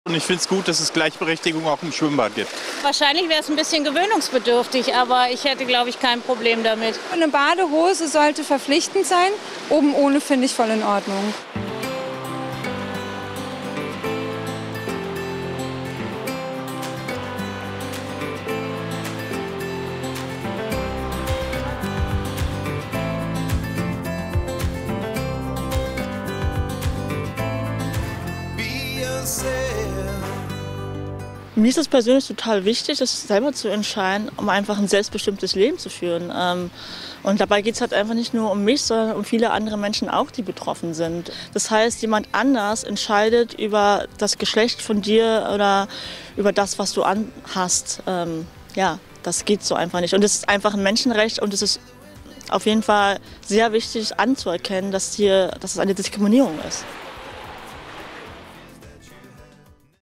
Oben ohne in Berliner Schwimmbädern - für alle
"Oben ohne" ins Schwimmbad ist ab sofort für alle Menschen in Berlin möglich. Eine Frau hatte sich bei der Ombudsstelle für Gleichbehandlung beschwert - und bekam Recht. Die Berliner Bäder haben nun ihre Badeordnung angepasst.